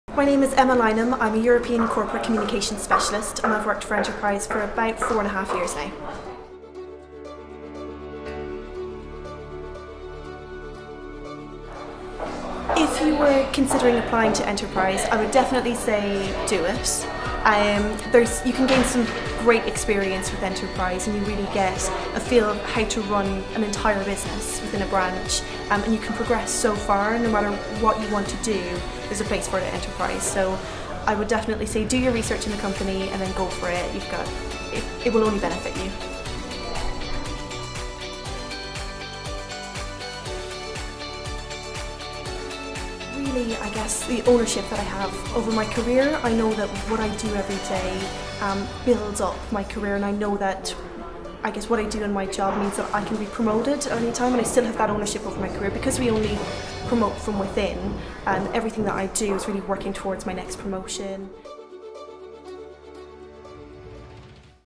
Emma Lynam - Enterprise Rent-A-Car Jobs
Find out what our European Corporate Communications Specialist has to say about life at Enterprise. For the latest Graduate and Placement opportunities with Enterprise Rent-A-Car